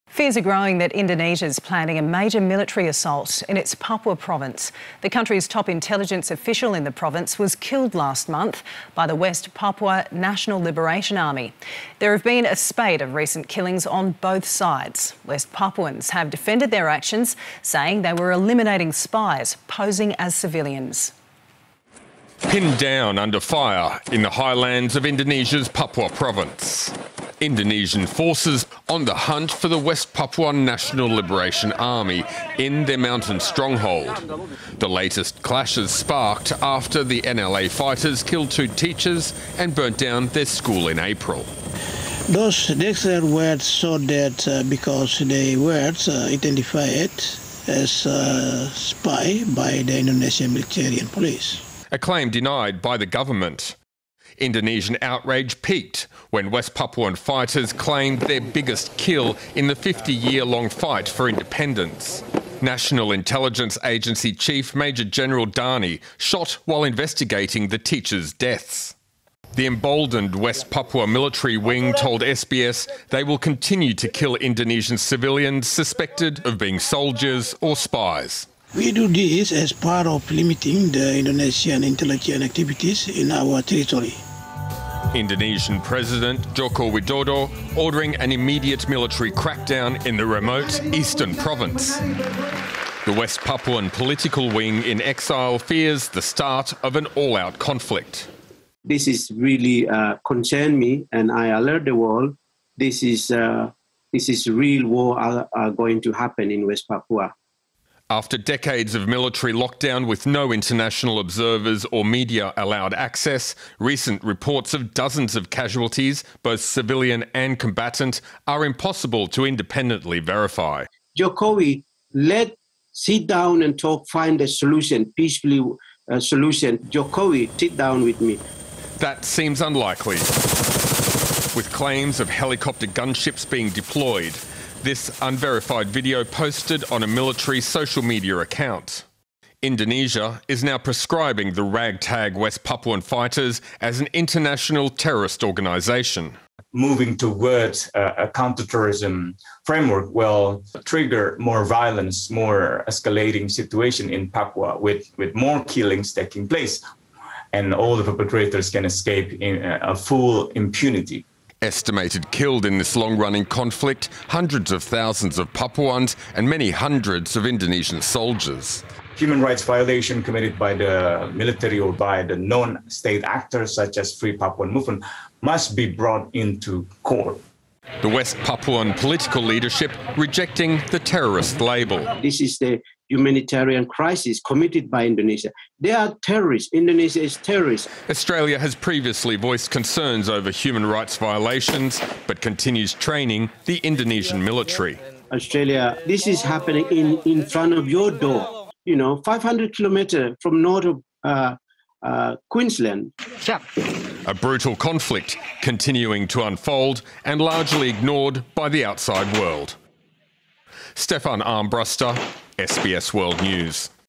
Stefan Armbruster reporting for SBS on West Papua conflict
Brutality in West Papua escalates with civilian and military casualties in the last month. The killing of Indonesia’s spy chief on the ground and proscribing of the independence movement and fighters as terrorists has amped up fears a major confrontation is looming.

The conflict continues to go largely unreported in Australia apart from on SBS News. Features WPNLA’s spokesman Sebby Sambom, ULMWP’s interim president Benny Wenda and Amnesty International Indonesia’s Usman Hamid.